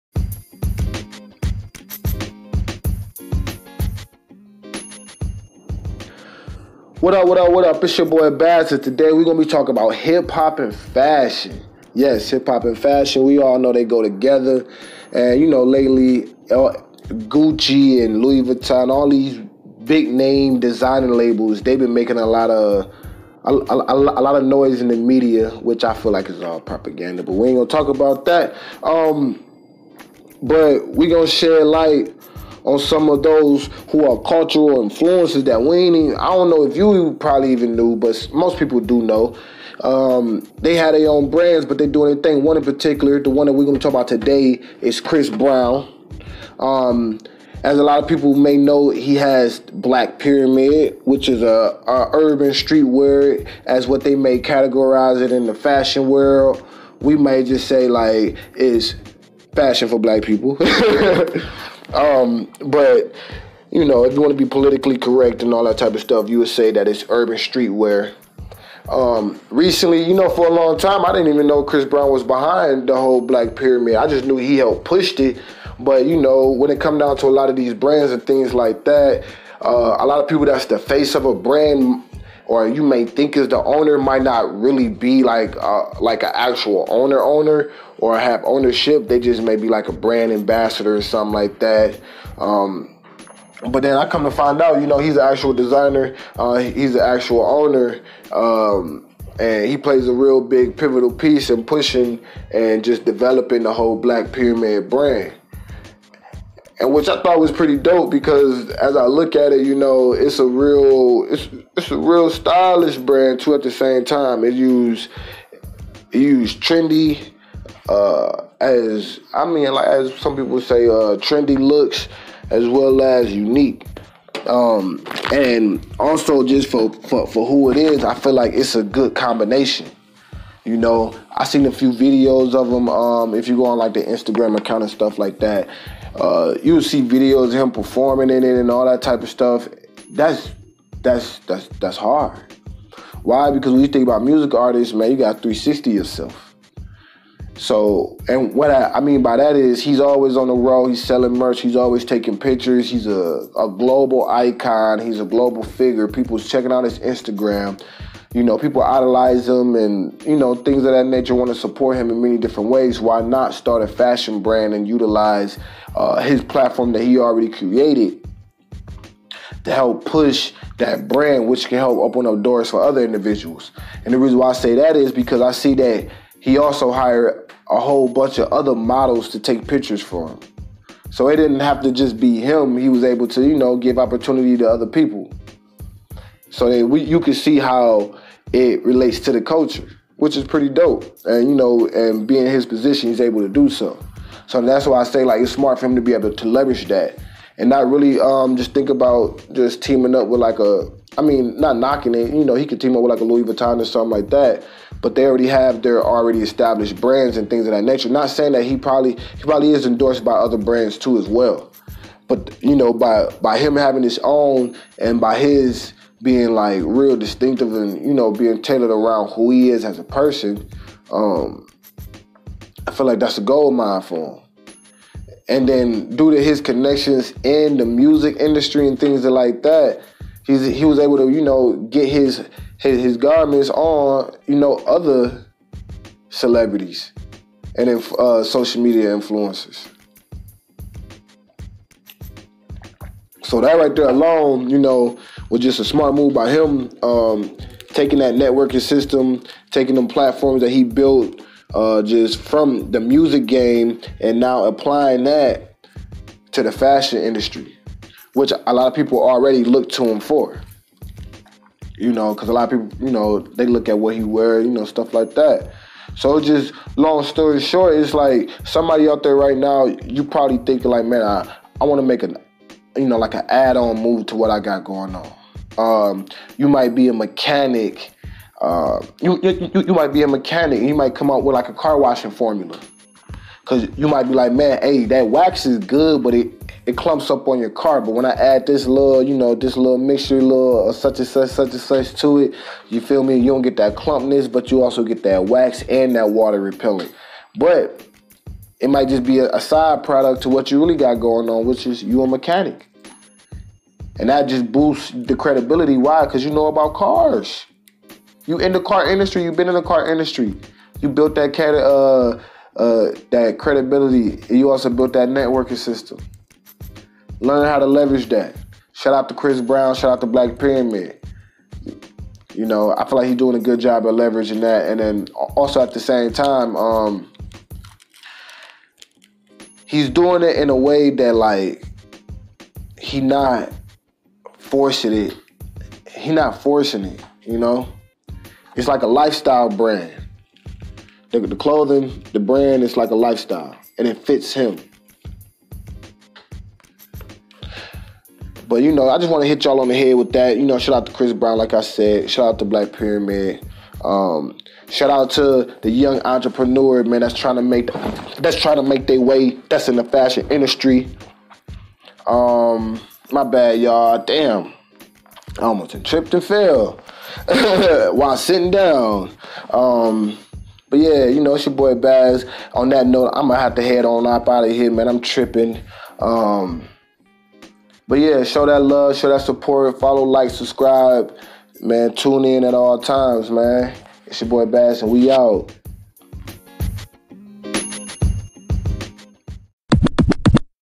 Chris Brown, Designer and Founder of Black Pyramid Clothing | Fashion Influence
In this episode Bazz discusses Chris Brown, singer and dancer in a different light as a entrepreneur and fashion designer. The Grammy winner launched and serves as a designer for his fashion company & brand Black Pyramid line since 2012. Joining partners Pink Dolphin founder, Cena Barhaghi and Neima Khalia to build an iconic brand.